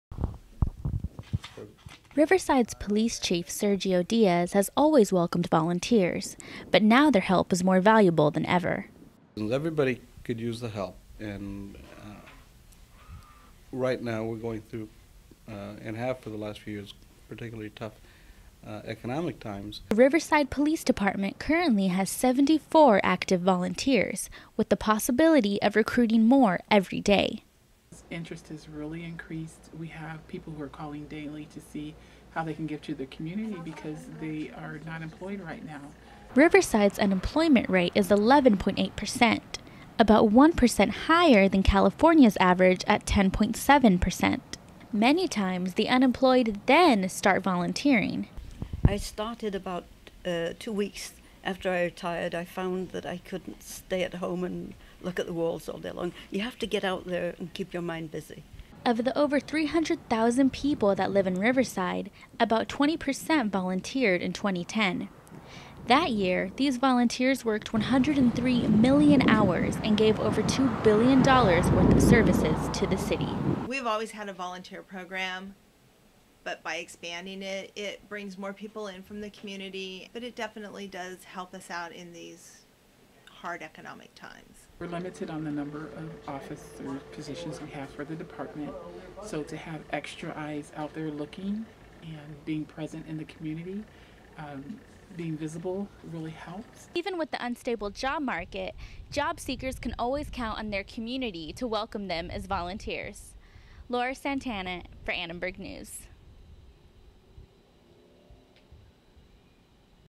Riverside Police Department Volunteers
By Laura Santana

Unemployment and budget cuts lead to a rise in volunteers. The spotlight is on the Riverside Police Department in this day-of-air story.